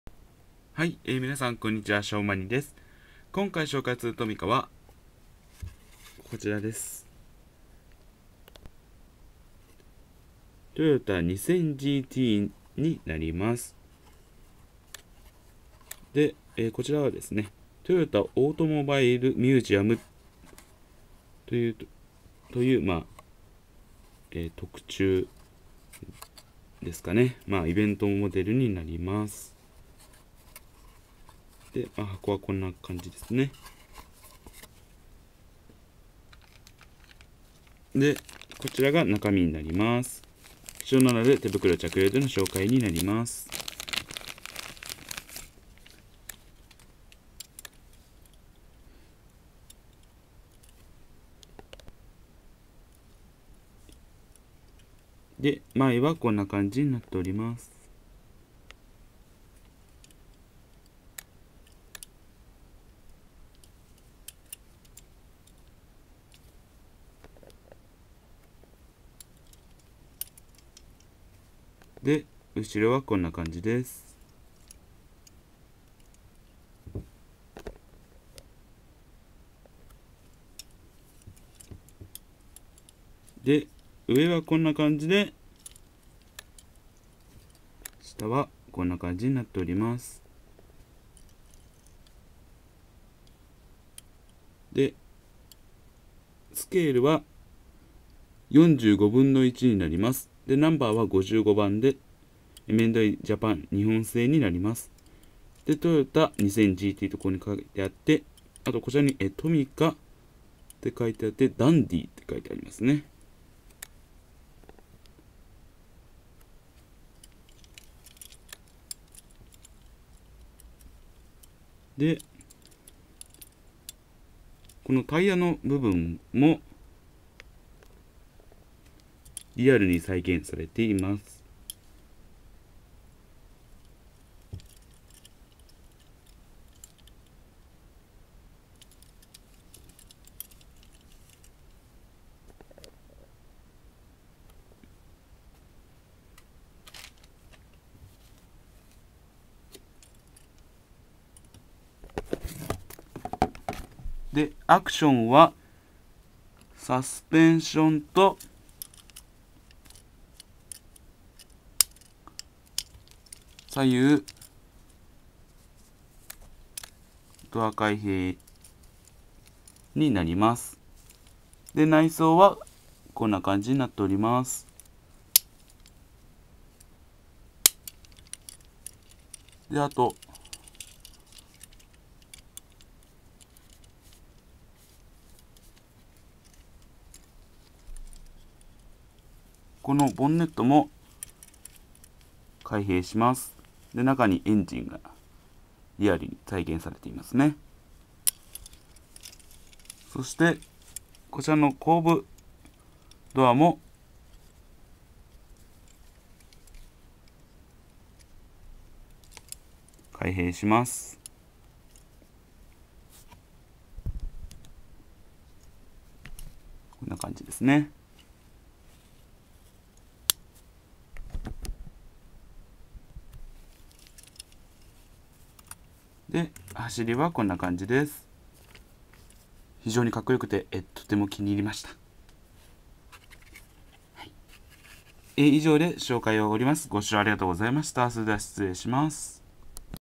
【トミカ開封 338】TOYOTA AUTOMOBILE MUSEUM 「TOYOTA 2000GT」 《日本製・超激レア・貴重》
開封 《日本製・超激レア・貴重》

・全てリアルに再現されている「トヨタ 2000GT」です＾＾
おそらく特注かイベントモデルだと思います・・・＾＾；
めっちゃカッコいい♪
お気に入りのトミカは沢山有りますが、これが一番気に入っています＾＾

※かなり貴重なトミカなので、手袋着用です・・・＾＾；